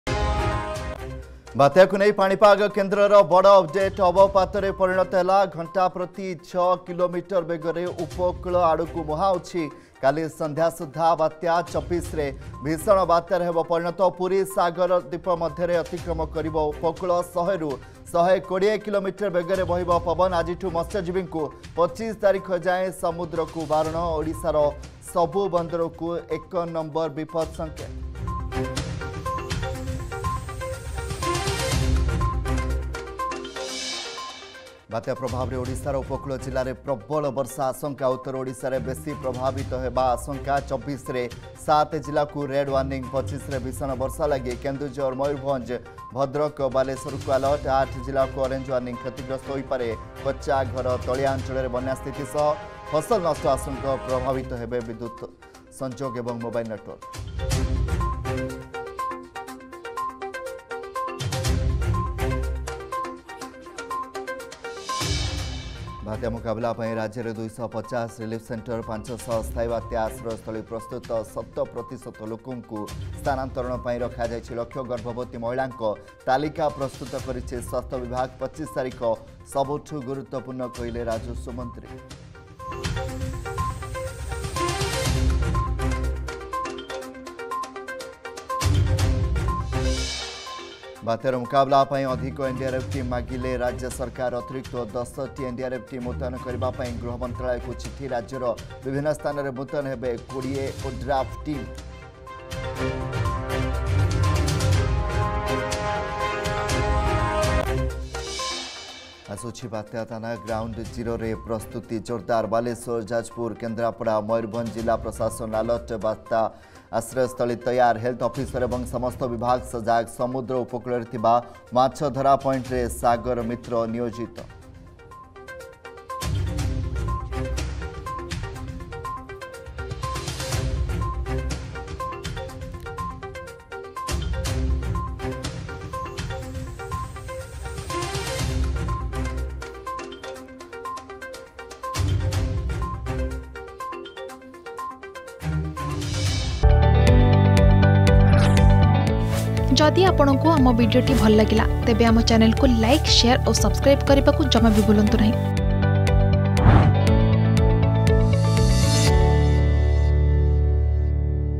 Headlines 2pm || 22 Oct 2024 || Argus News
+ବାତ୍ୟାକୁ ନେଇ ପାଣିପାଗ କେନ୍ଦ୍ରର ବଡ଼ ଅପଡେଟ୍
+ବାତ୍ୟା ପ୍ରଭାବରେ ଓଡ଼ିଶାର ଉପକୂଳ ଜିଲ୍ଲାରେ ପ୍ରବଳ ବର୍ଷା ଆଶଙ୍କା
+ ବାତ୍ୟାକୁ ନେଇ ବିଭିନ୍ନ ମଡେଲର ଆକଳନ
+ବାତ୍ୟା ମୁକାବିଲା ପାଇଁ ରାଜ୍ୟରେ 250 ରିଲିଫ ସେଣ୍ଟର ଓ 500 ଅସ୍ଥାୟୀ ବାତ୍ୟା ଆଶ୍ରୟସ୍ଥଳୀ ପ୍ରସ୍ତୁତ
+ସତ ହେଲା ପାଞ୍ଜିକା କଥା
+ଆସୁଛି ବାତ୍ୟା `ଦାନା'